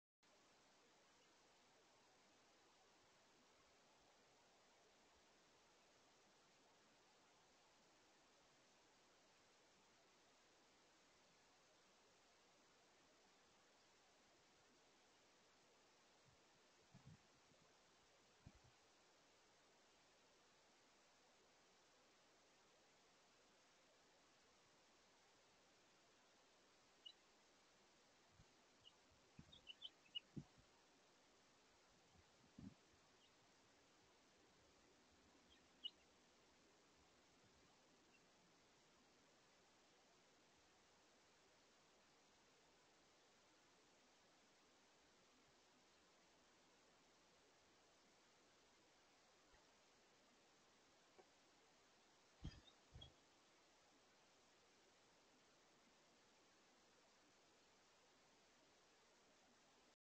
2023/04/02 - House Finch couple are enjoying dinner until a female Northern Cardinal arrives
I think the House Finches would have stayed if the female Cardinal would have settled in.  It has been a long time since I have seen a Cardinal do the grab-n-go routine at my feeder.  She must be wary of the male Cardinal that has been bullying her around as of late.  I hope you enjoyed the video.